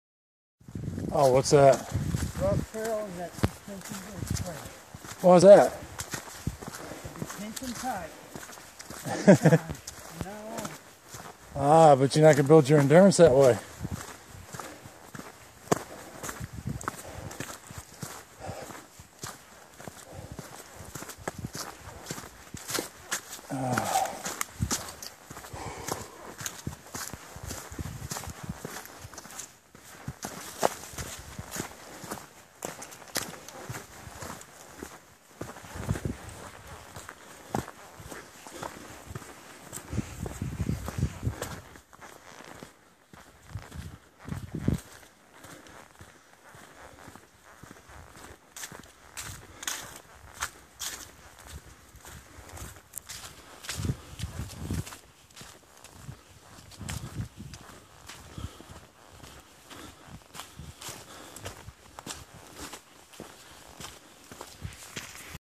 Pinch'em Tight Trail RRG, KY
The easiest way I have found to Hanson's Point.